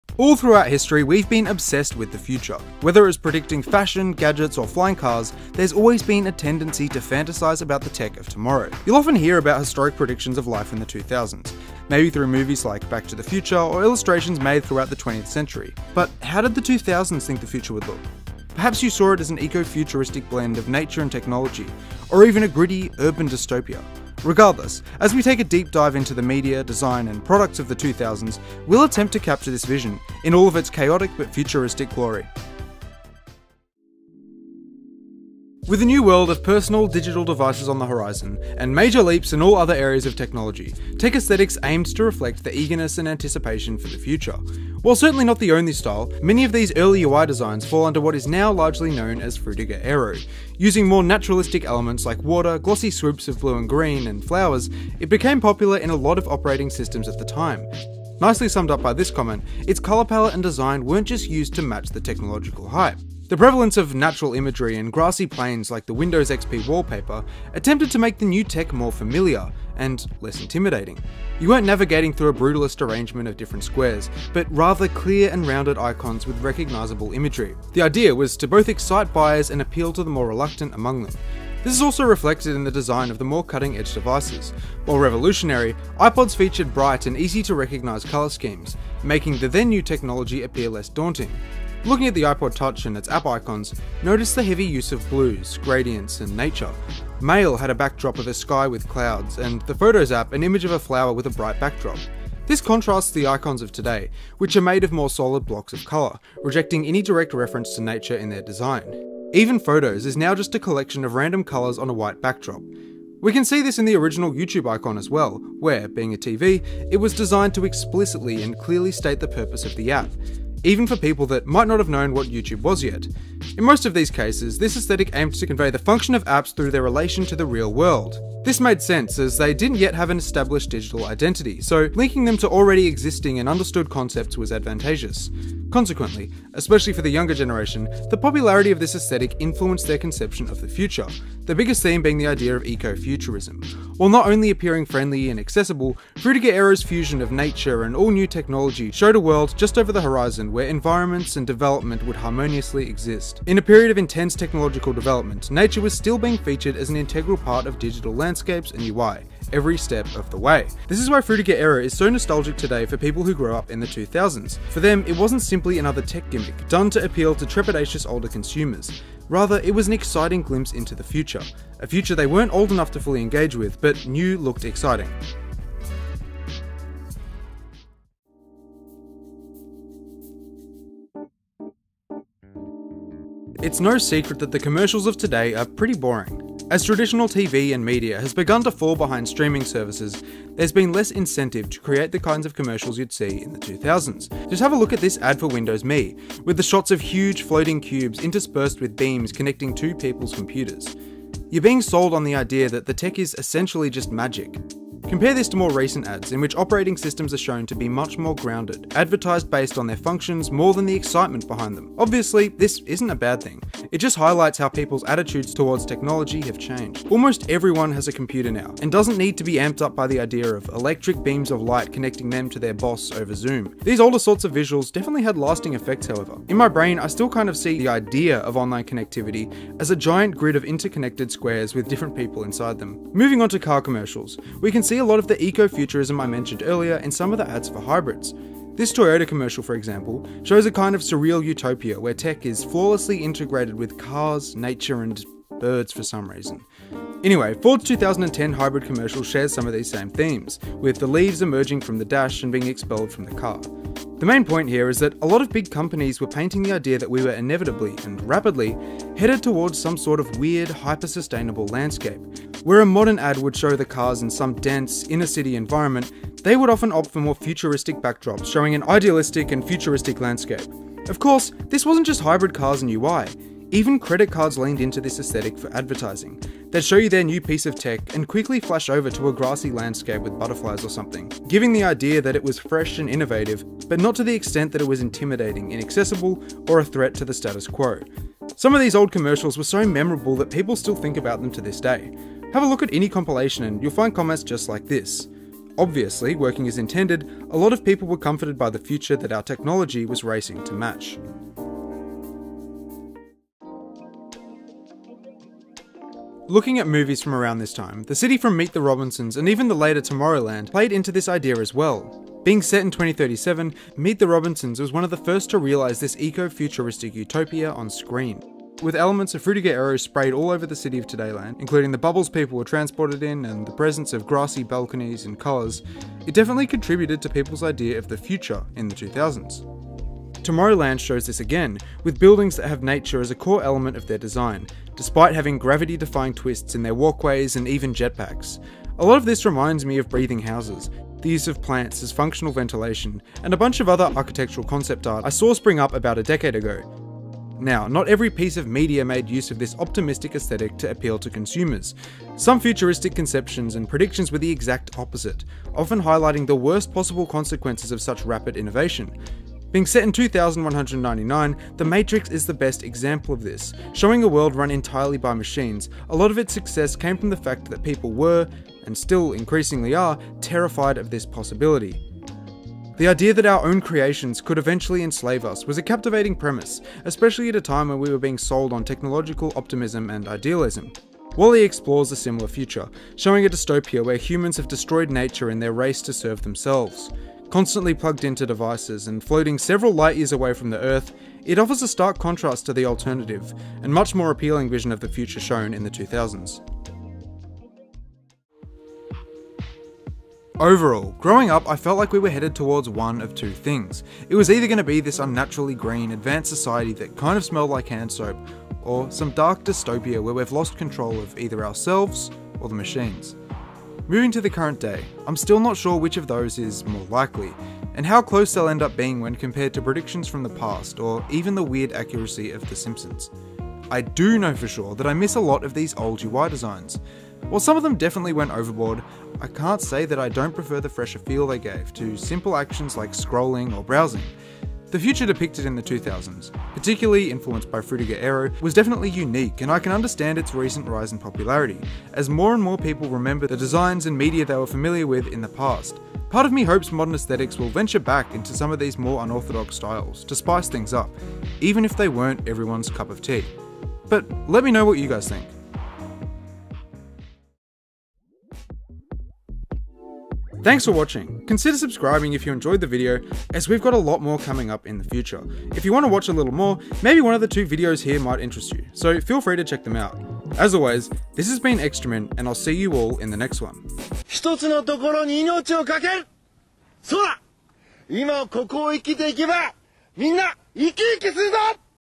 The Future Felt Different in the 2000s…
Looking at Frutiger Aero and many of the movies and products of the 2000s, we can begin to get an idea of the 'future' that many believed we were headed towards.

CONTENT
0:00 Intro
0:34 A Digital Aesthetic?
2:58 Frutiger Aero and TV
5:03 Movies of the Future
6:40 My Thoughts
7:40 Conclusion